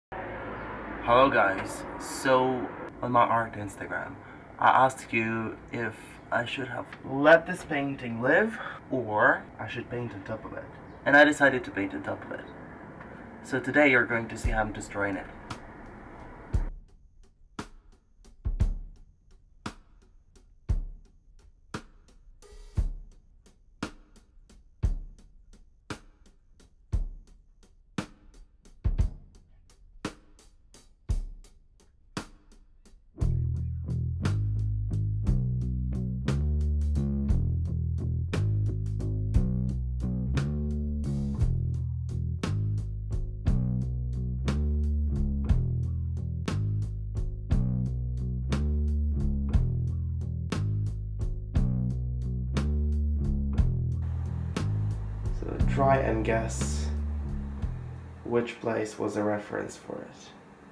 This is what happens when you hate your painting
I ran a poll on my art Instagram ) asking whether I should leave the portrait alone or paint over it. And eventually I decided its life came to end... (evil laughter)

Here's my website if your'e curious to see what I do ;)

Have a fun day! :D